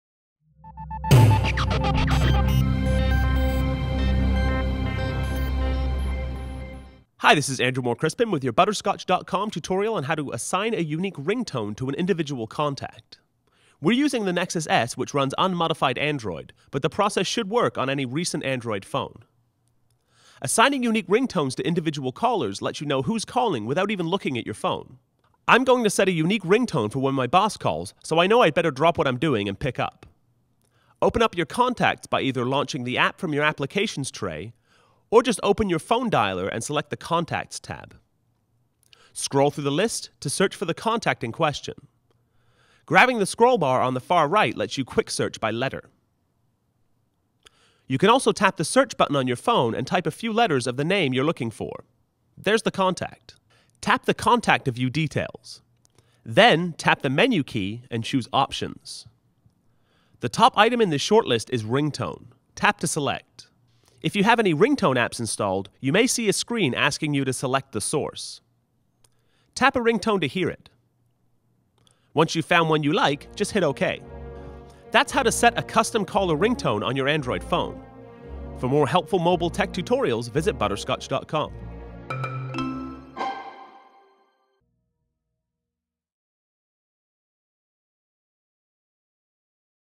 Assigning contact specific ringtones on your Android
An easy way to find out who's calling you without even looking at your phone is to assign custom ringtones to your contacts. On Android smartphones it's a fairly simple process, as we'll show you in this tutorial @ butterscotch.com